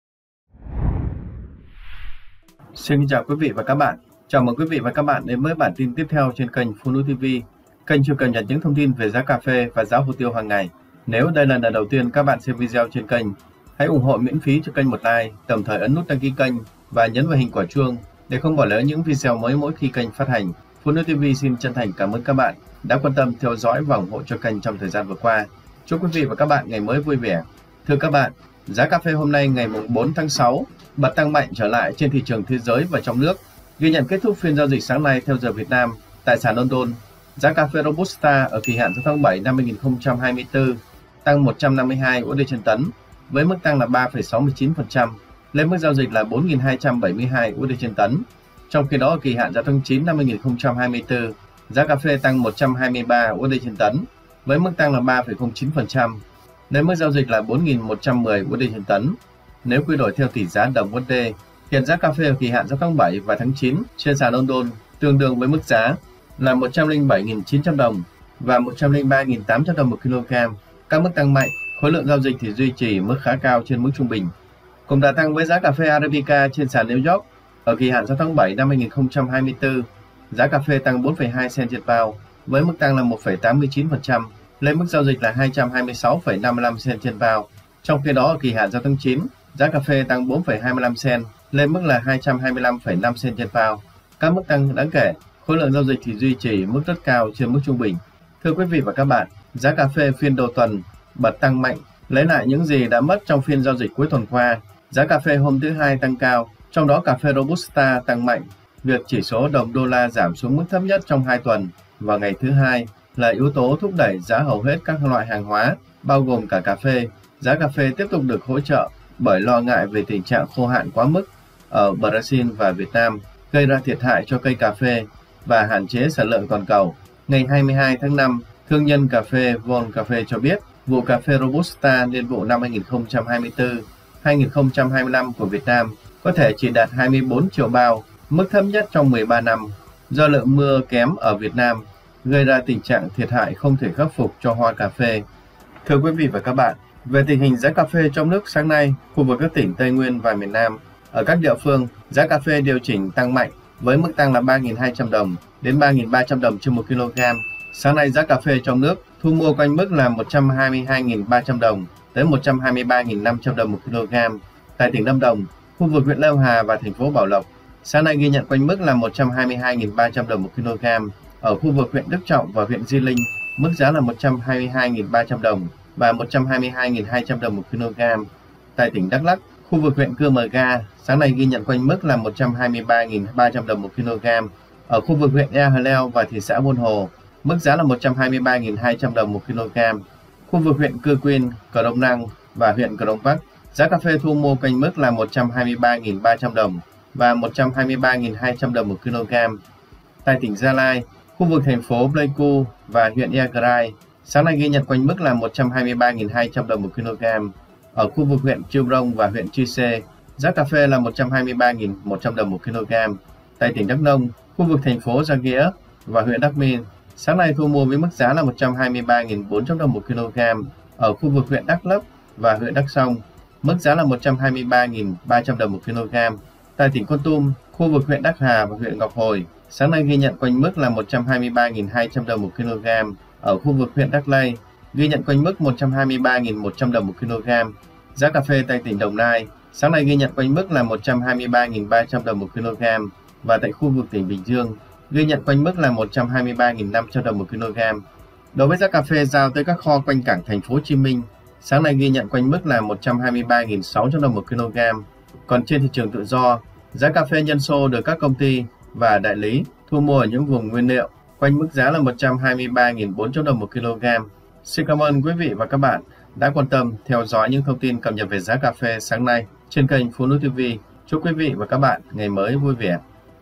Giá Cà phê hôm nay ngày 4/6/2024|Giá cà phê TĂNG PHI MÃ đồng loạt trở lại
👉 Bản tin cập nhật giá cà phê, hồ tiêu hôm nay ngày 4/6/2024 được chúng tôi cập nhật chi tiết, các thông tin trên thế giới và trong nước tại các vùng trồng trọng điểm khu vực Tây Nguyên và miền Nam. Giá chi tiết tại các địa phương như Đắk Lắk, Gia Lai, Kon Tum, Đắk Nông, Lâm Đồng, Đồng Nai, Bình Dương...
© Copyright by PhoNui TV ☞ Do not Reup